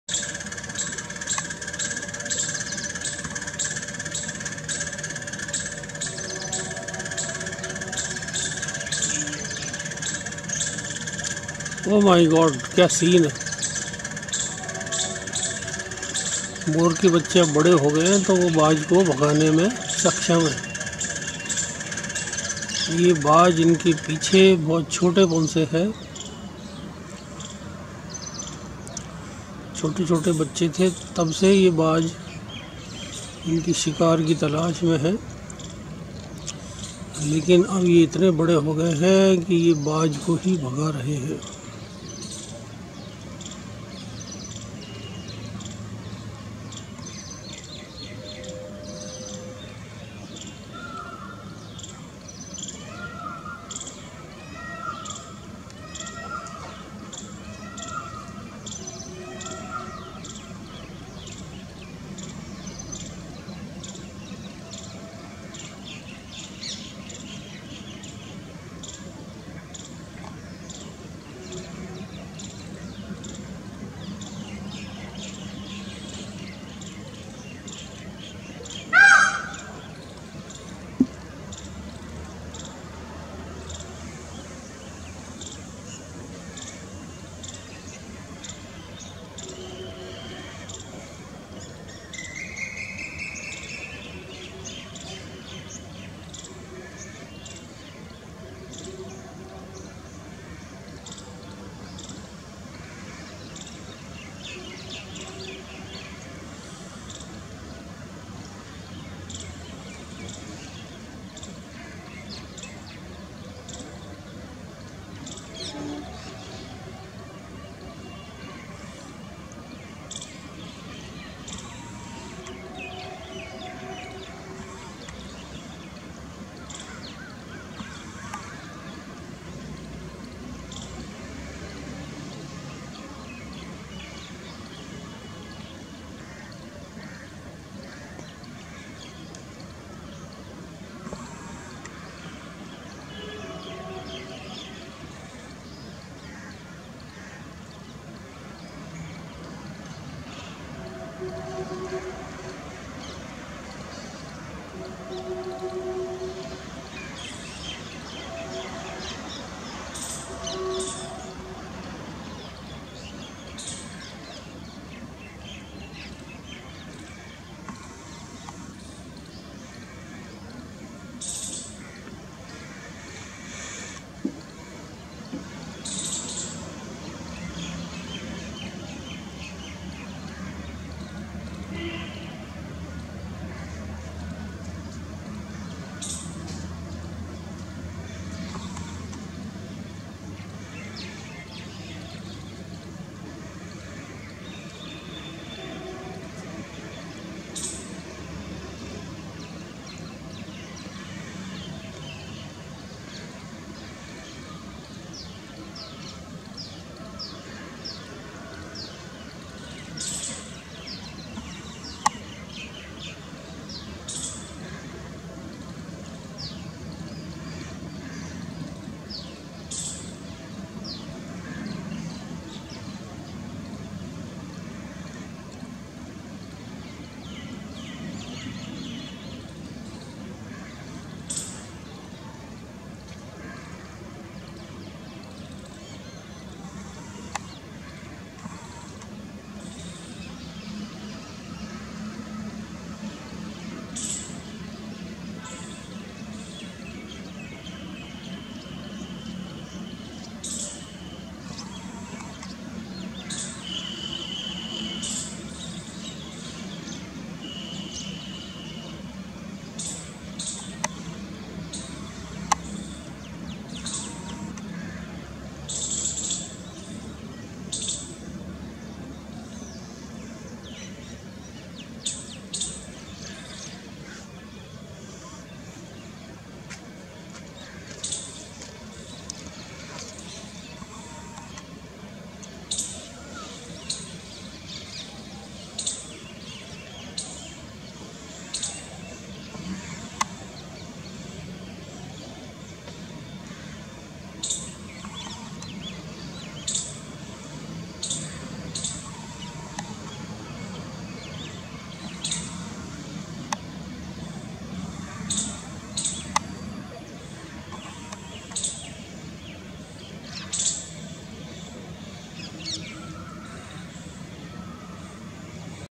मोर के बच्चे में अब बाज़ Shikra को भगाने की हिम्मत आ गई ||अभी तक बाज़ मोर के बच्चों का शिकार करता था
Mor ke bachhe ab bade ho gaye hai aur ab inme baaz ko bhagane ki himmat aa gayi hai.
Jab bachhe chote thy tab baaz in bacchon ka shikar ker liya kerta tha.badi mushkil se mor ke ye  bachhe bade hue hain.to ab aap dekh rahe hain ki ek mor ka baccha baaz ko bhagane me laga hua hai.
Enjoy Nature.